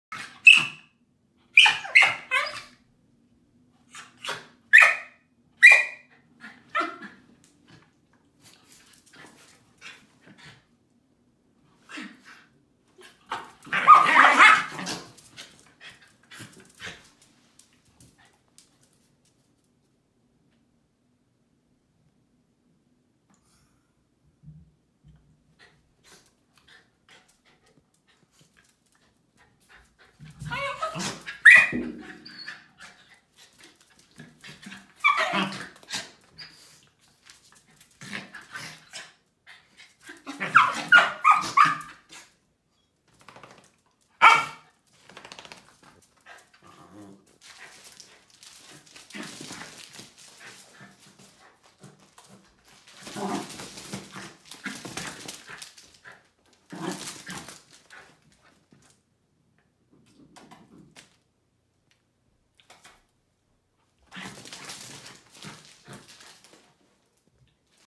Tidewalker Australian Terriers
Kanti and Sisu hamming it up
CanGch AmCh Dreamtime’s Bollywood Kanti CGN CAA RN (Dam of Sisu) age 8 years
Tidewalker Seas The Day “Sisu “ age 9 months